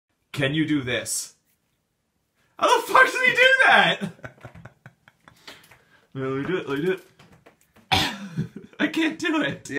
Is your Hand Evolved Enough To Do This
Is your hand evolved enough to do this? Do you have a Palmaris Longus? My Friend is part of 14% of the Human Population who have evolved to no longer need a Palmaris Longus. I still have one and it prevents me from doing that.